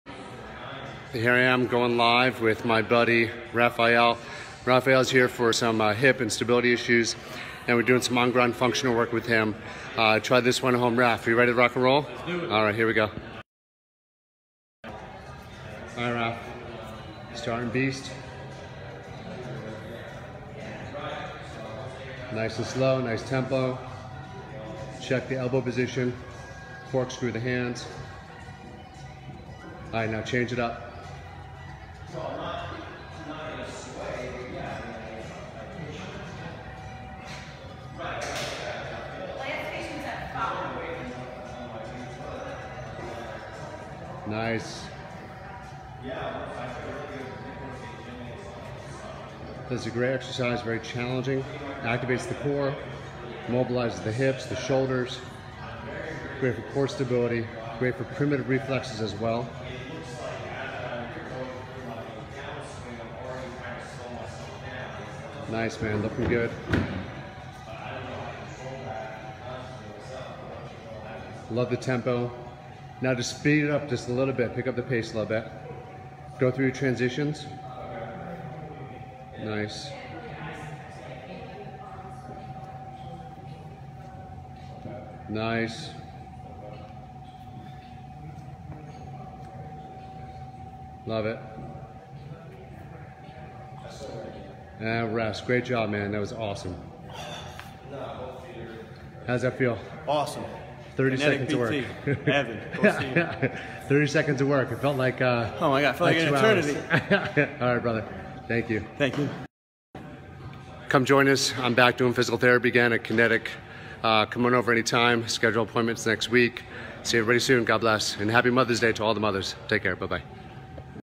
How to Improve Hip & Stability Issues with Advanced Core Activation Training
Evan Chait of Kinetic PT shows exercises to do for advanced core training.

Looking to take your health to the next level? Take one of our life changing courses at: